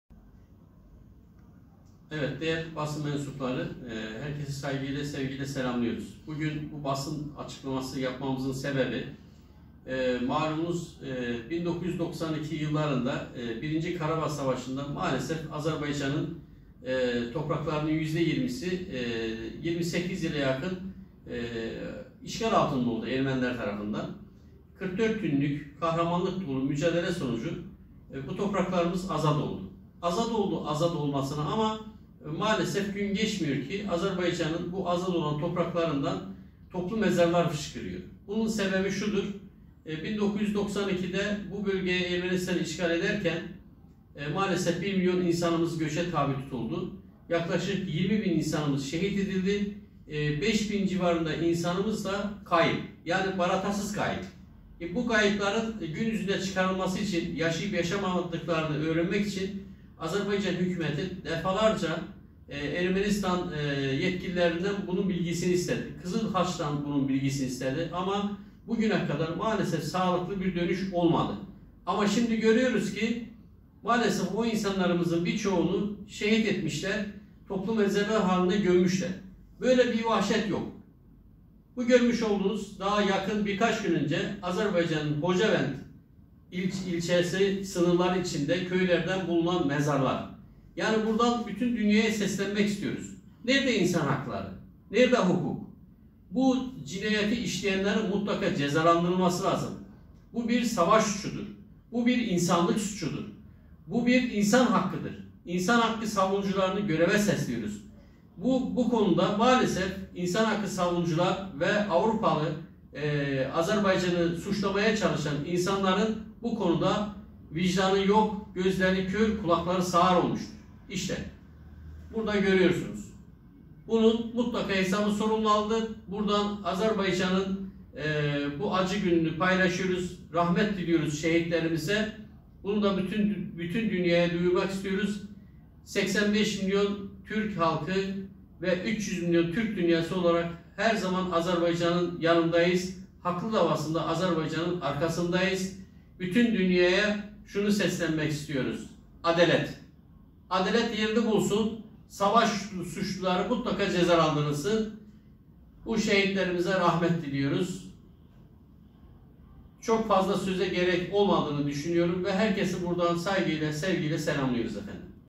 Hocavend'de Ermeni Vahşeti
Her gün, yeni bir Ermeni vahşetiyle karşılaşıyoruz. Azerbaycan’ın Hocavend şehrinin,Edilki köyünde, ‘Birinci Karabağ Savaşı’ esnasında, Ermeniler tarafından katledilen 12 Azerbaycanlıya ait yeni bir toplu mezar tespit edildi. Birinci Karabağ Savaşı’nda kaybolan insanların bulunması için yapılan araştırmaların ve şahitlerin ifadeleriyle, toplu mezarlıkların bulunduğu belirtildi. Konu ile ilgili basın açıklaması yapan İSTAD Başkanı Sefer Karakoyunlu; ”İkinci Karabağ savaşında 44 günlük kahramanlık dolu mücadele sonucu, topraklarımız işgalden azad oldu. Ancak bizim 1. Karabağ savaşında, 20 bin civarında şehidimiz, bir milyon göçe tabii tutulan halkımız, Hocalı’da yapılan soykırımda da yaklaşık 613 kişi bir gecede katledildi ayrıca nerde olduğu bilinmeyen 5000 kaybımız var. Defalarca Ermenistan resmi kurumlarına ve Kızılhaç teşkilatlarına başvurulduğu halde, kayıpların akıbeti hakkında bu güne kadar bilgi alınamadı. Her gün Azerbaycan’ın işgalden azad edilen bölgelerinden toplu mezar fışkırıyor. Dünya neden sessiz kalıyor. İnsan haklarından dem vuranlar, sağır ve körü oynuyor. Bu duruma anlam vermekte zorluk çekiyoruz. Bu bir savaş ve insanlık suçudur. Bunu yapanların mutlaka cezalandırılması gerekir. Soydaşlarımızın kanı yerde kalmamalıdır” dedi.